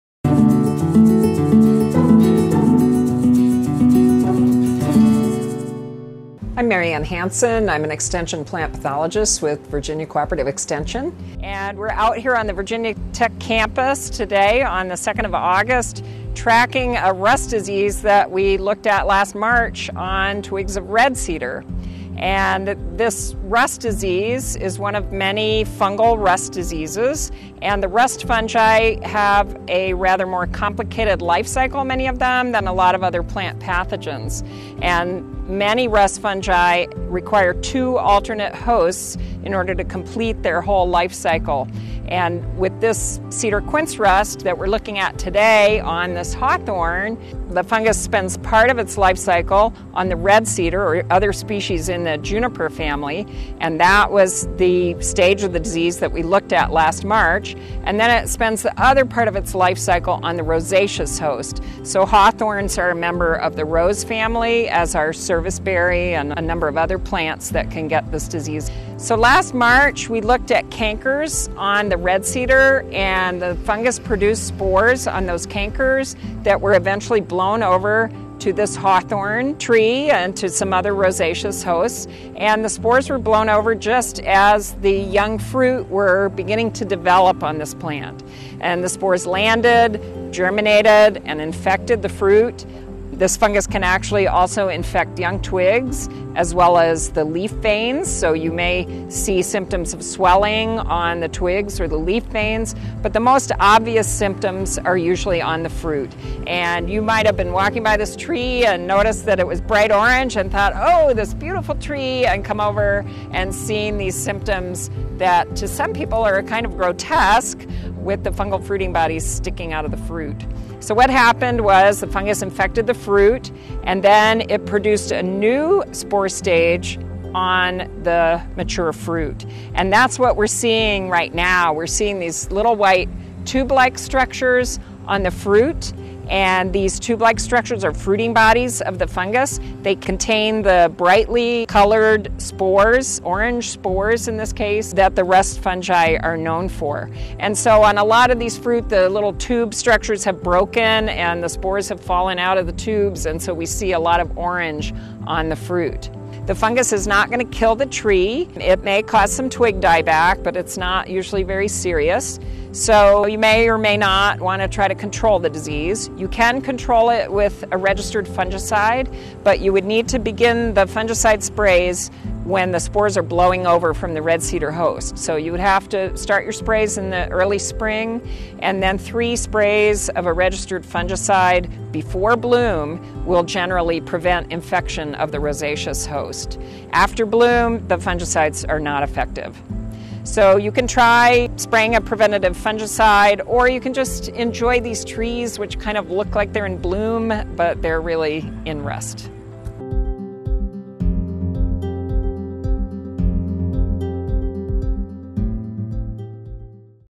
Cedar Quince Rust on Hawthorn - Common Plant Diseases in the Landscape and Garden
Join Virginia Tech Plant Pathologist, Mary Ann Hansen, as she discusses common plant diseases in Virginia. In this video she talks about cedar-quince rust on hawthorn.

For more information on the Virginia Cooperative Extension Master Gardener Program